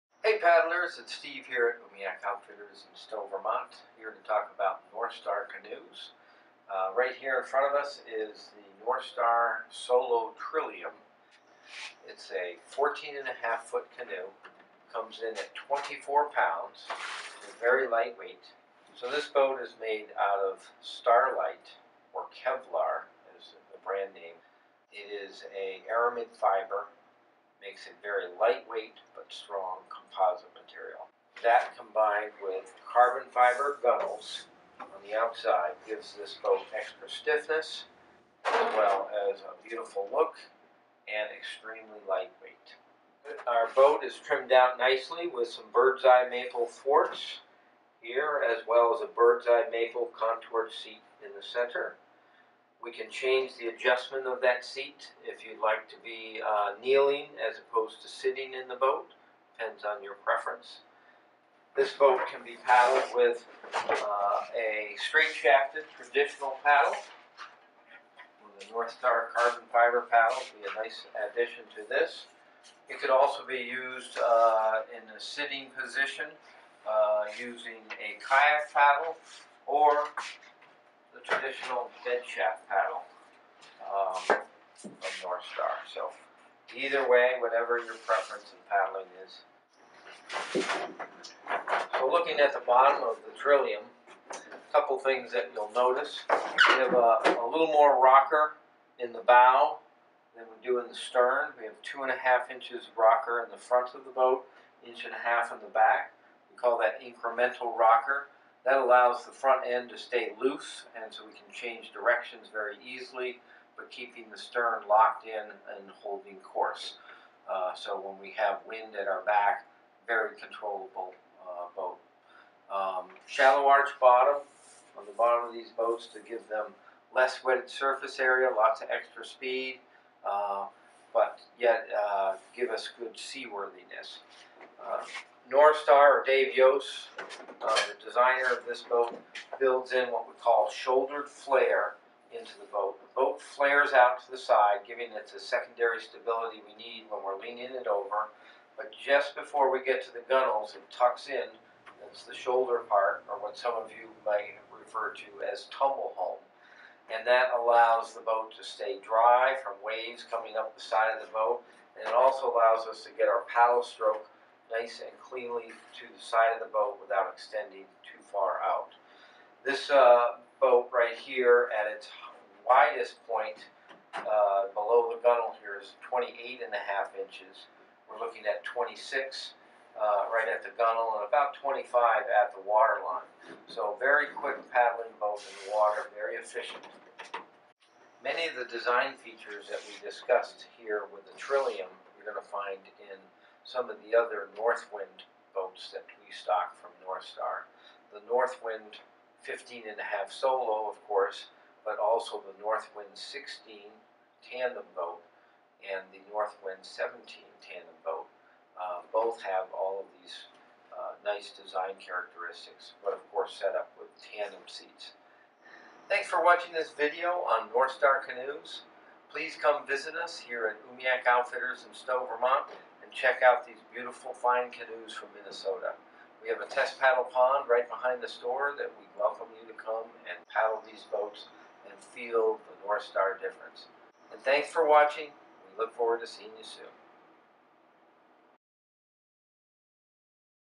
Northstar Canoe - Trillium
A do everything canoe with a performance oriented hull for those who want to paddle everywhere from lakes to rivers.

READ MORE!!

Umiak Outdoor Outfitters
Vermont's Outdoor Adventure Center
Stowe, VT & Richmond, VT